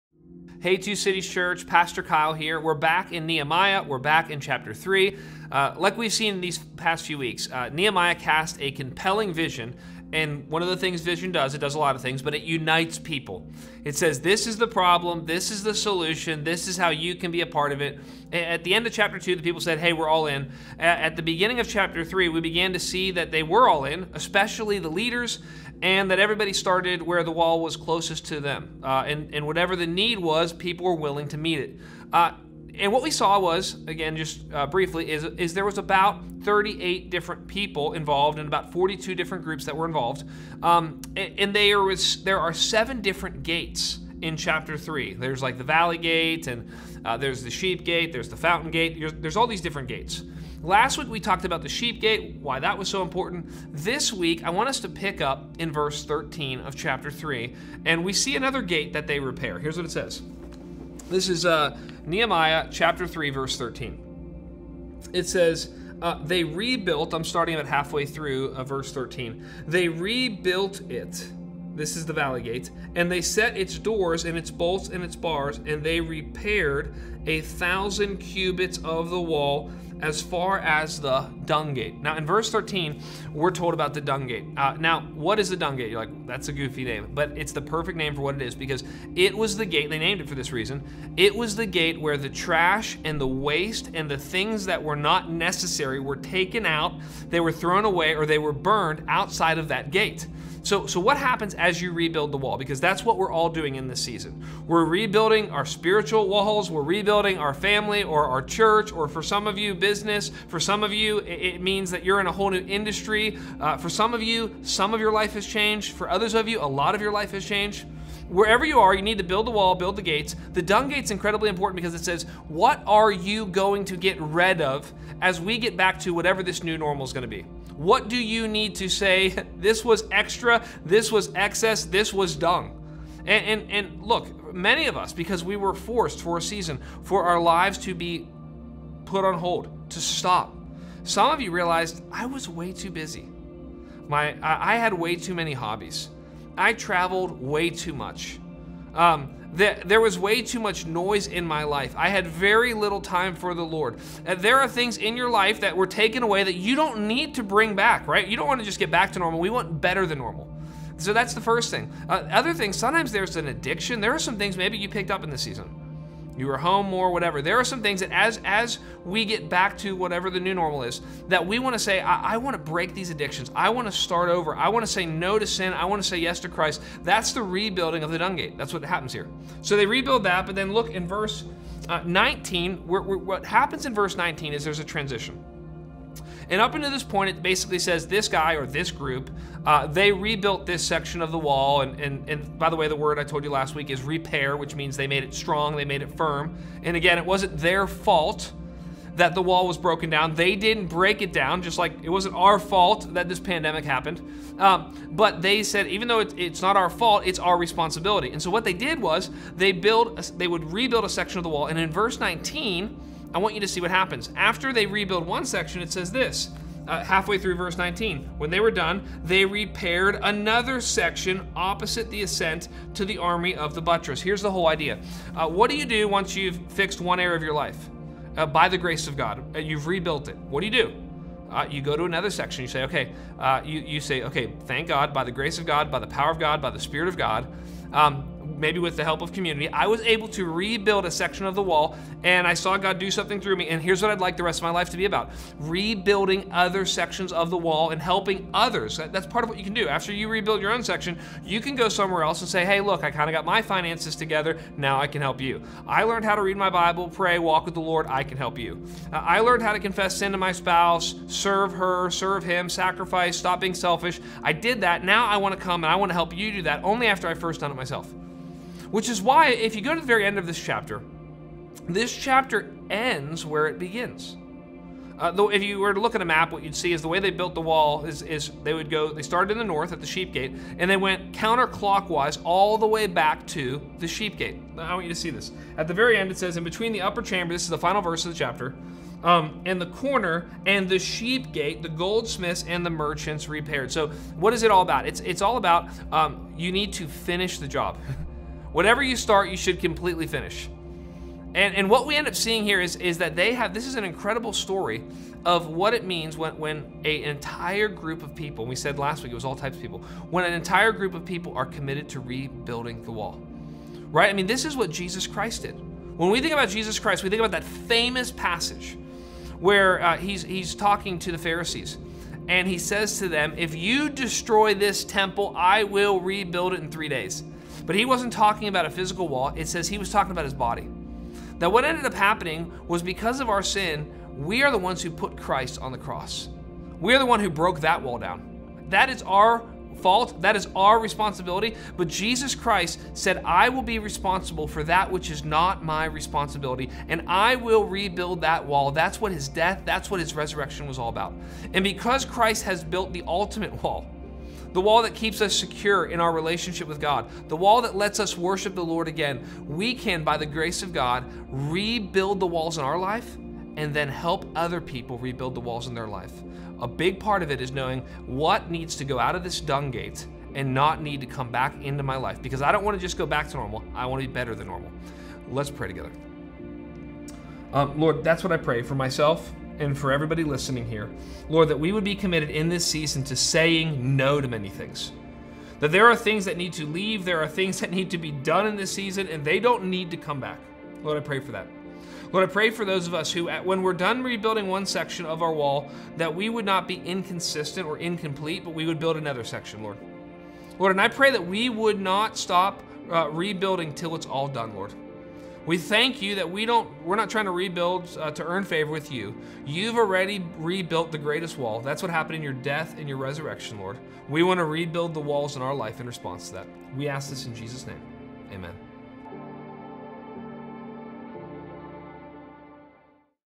Build it Better
Nehemiah 3:13-32 // Nehemiah shows us that before we rebuild, we must reevaluate. What areas of your life do you need to address? What excess do you need to cut out?